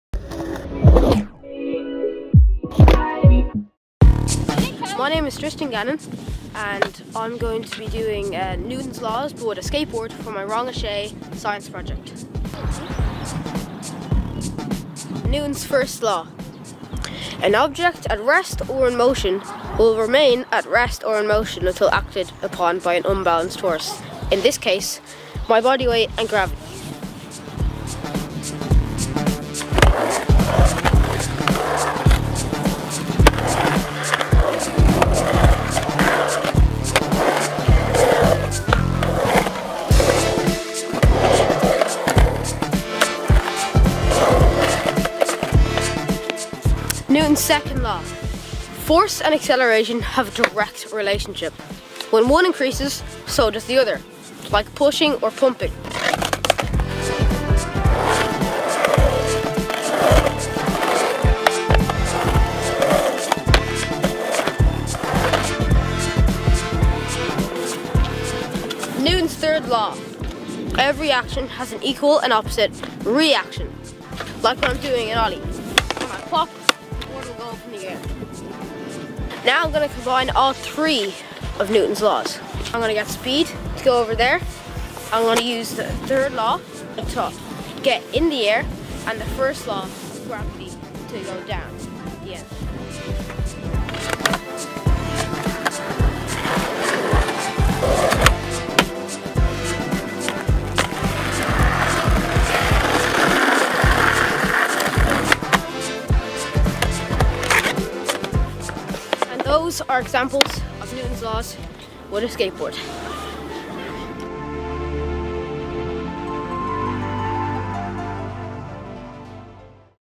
Newton's Laws of motion - on a skateboard
Newton's laws of motion, his first, second and third laws, can all be explained with a skateboard.
Tristan Gannon of Artkk Skateboards explains and demonstrates...
Science ROCKS!
Follow Tristan on instagram - Artkk Skateboards